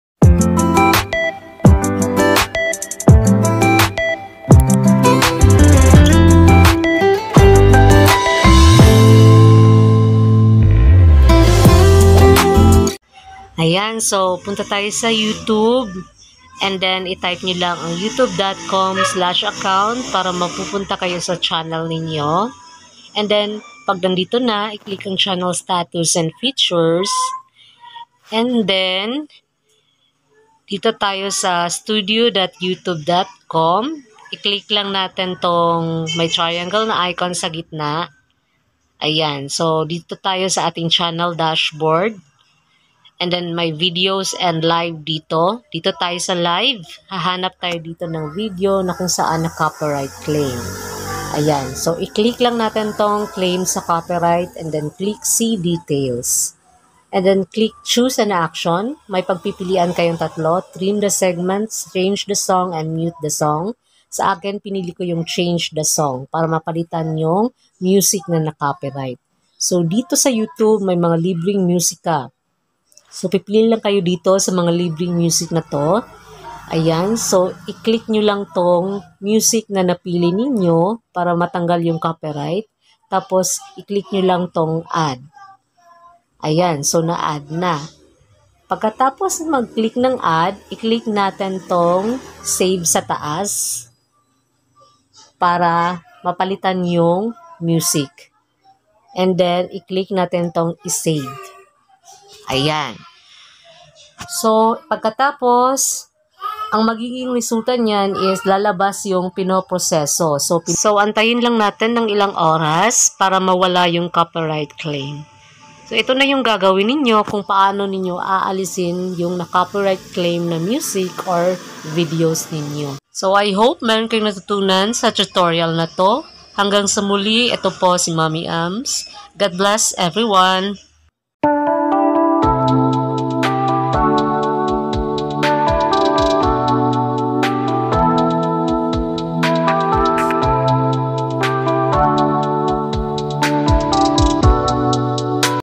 PAANO ALISIN ANG COPYRIGHT CLAIM SA YOUTUBE VIDEOS
Aralin natin kung PAANO ALISIN ANG COPYRIGHT CLAIM SA YOUTUBE VIDEOS